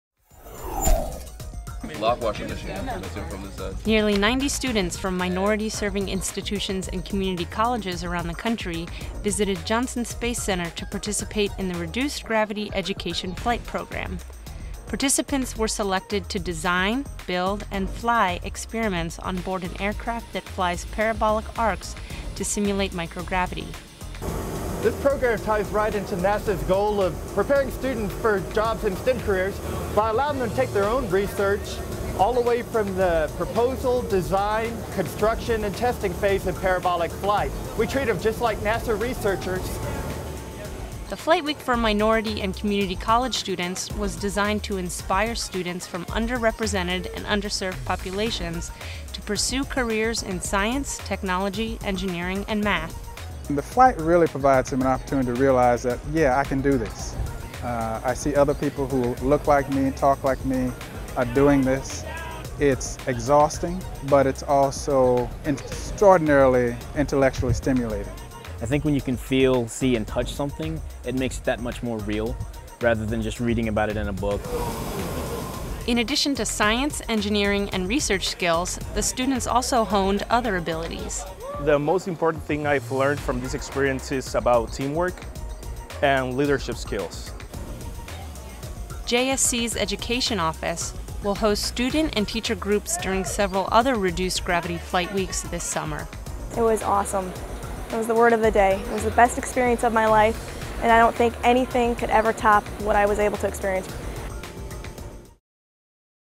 Students Test Experiments in Microgravity
Students from minority serving institutions and community colleges around the country participated in the Reduced Gravity Education Flight Program. Participants designed, built and flew experiments on board an aircraft that flies parabolic arcs to simulate microgravity.